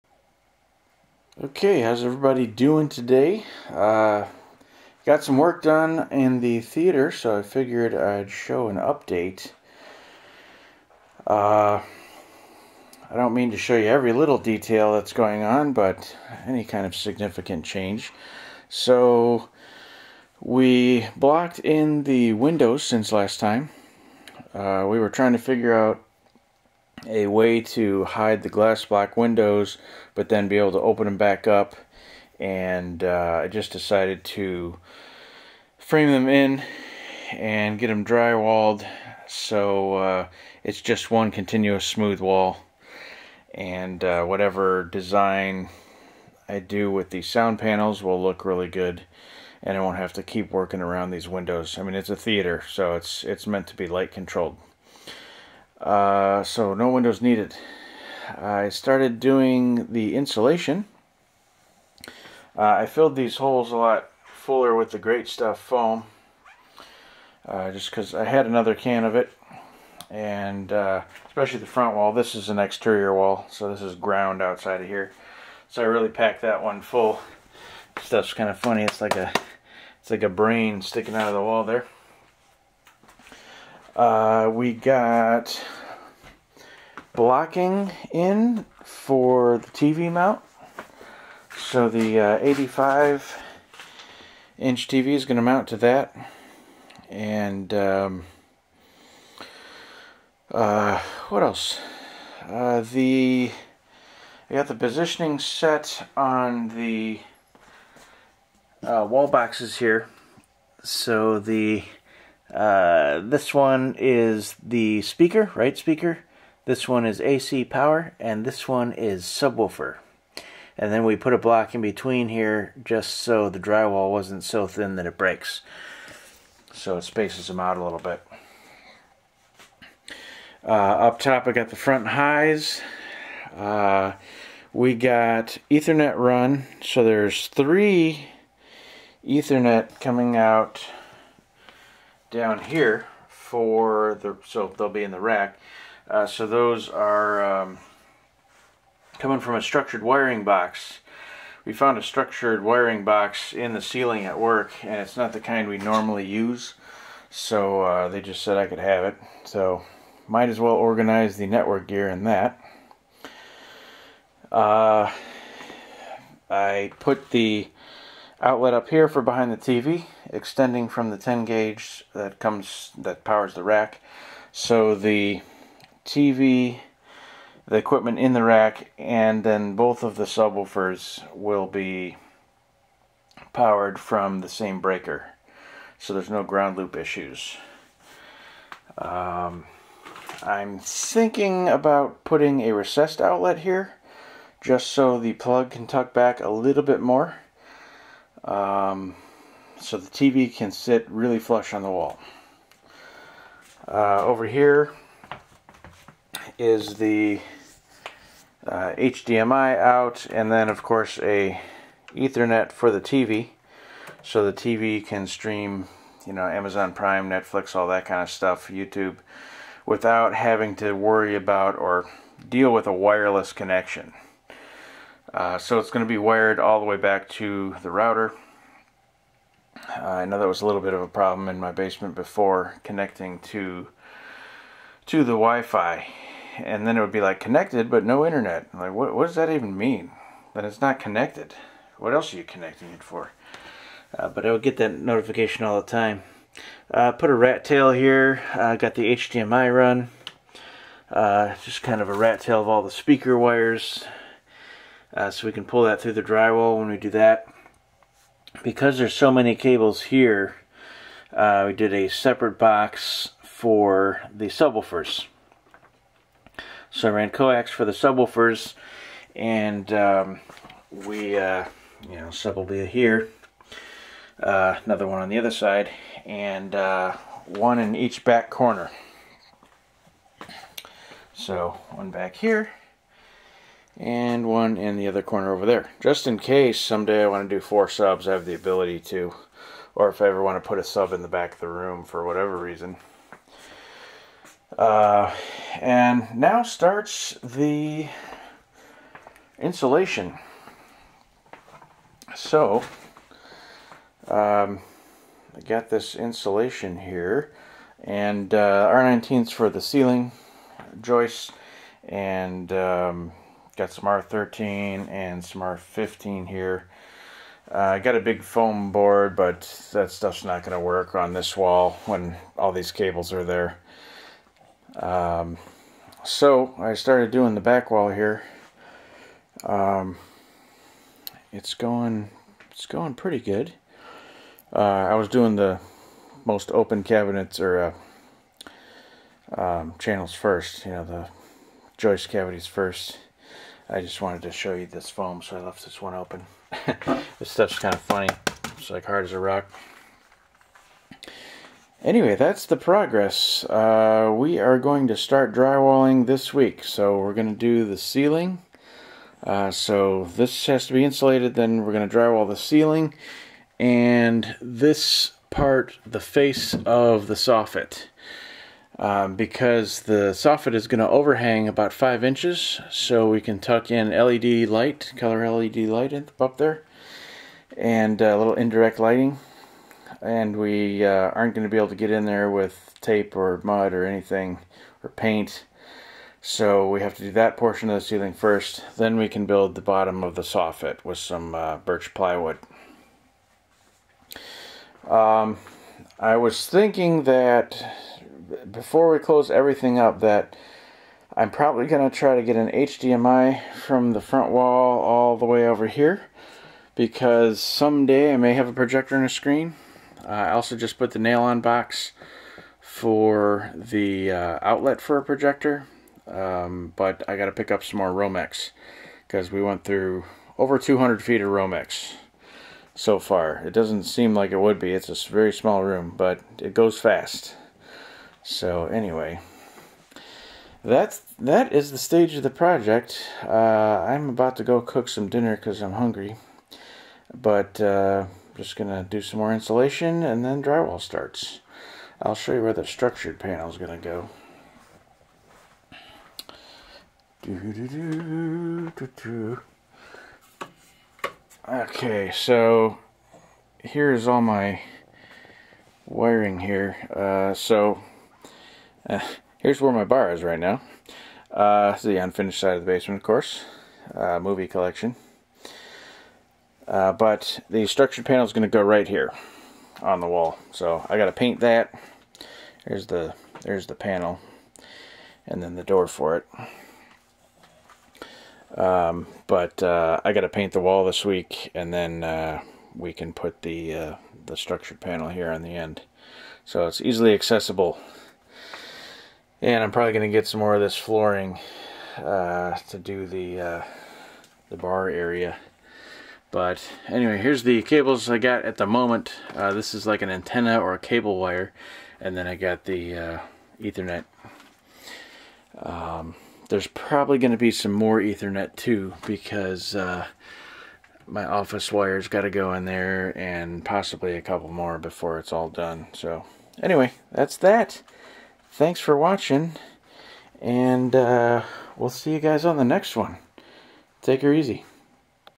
Home Theater Project Part 7. Glass block windows, more wiring, and insulation started.
Another update to the Home Theater project. Lot's of time is getting spent, but the progress is steady.